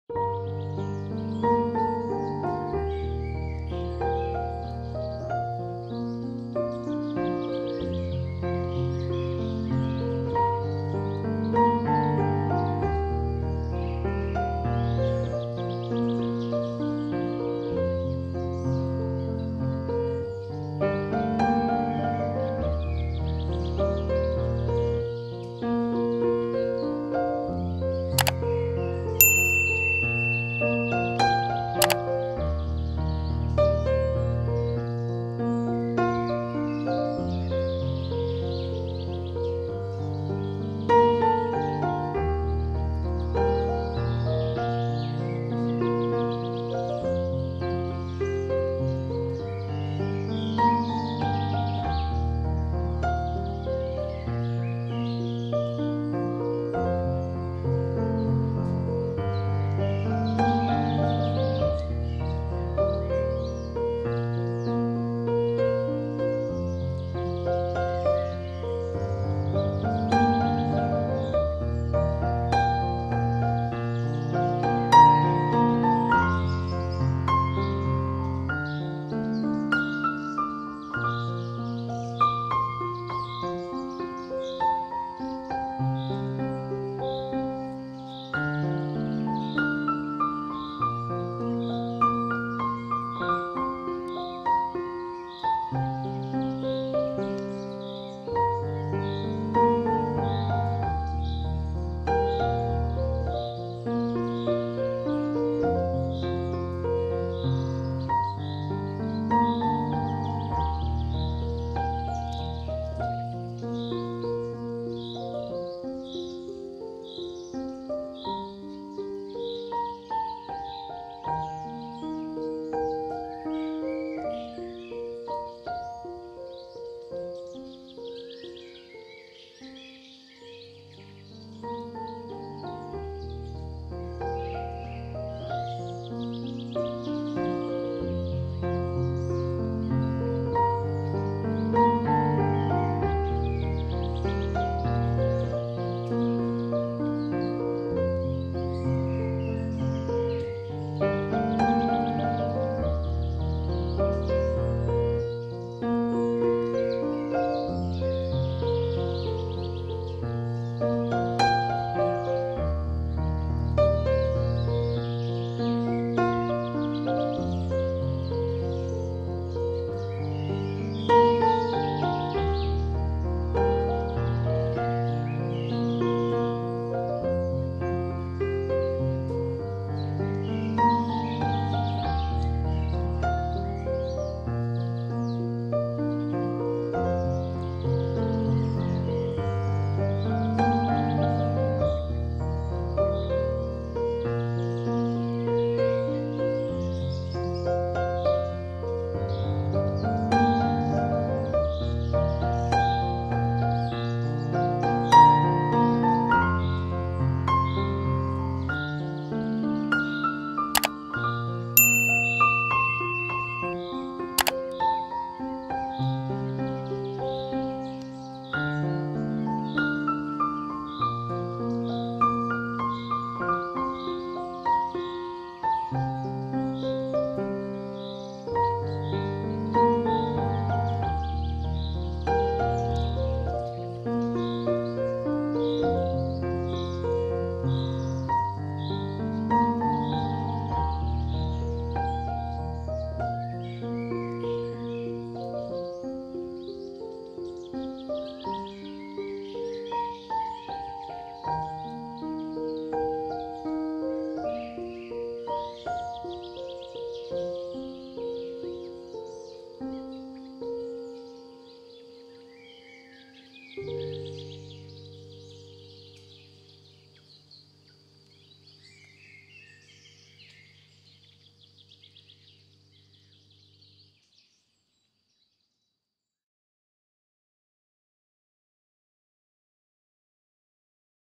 Relaxing Birdview Experience at Rakkiththakanda Rajamaha Viharaya | රක්කිත්ත කන්ද රජමහා විහාරය | 4K
Rakkhiththakanda Rajamaha Viharaya is an ancient cave temple rarely visited by the average traveller. The temple lies 2 km off the picturesque Ella – Wellawaya Road in the village known as Karandagolla in the Wellawaya DS area of Monaragala District.